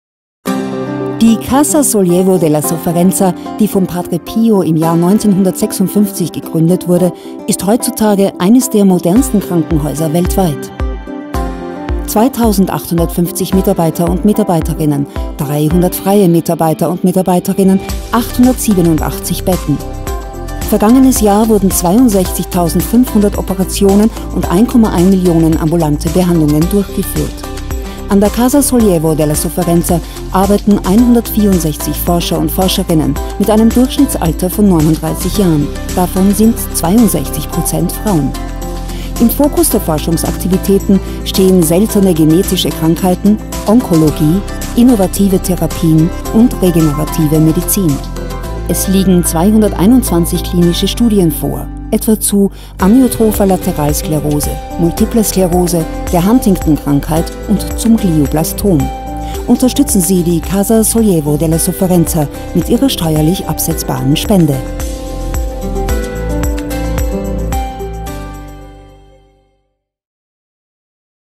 Die Casa Sollievo della Sofferenza
Die Casa Sollievo della Sofferenza, gestiftet im Jahr 1956 von Padre Pio als Haus zur Linderung des Leidens, ist ein Krankenhaus und medizinisches Forschungszentrum spezialisiert auf die Bereiche genetische Erkrankungen, innovative Therapien und regenerative Medizin.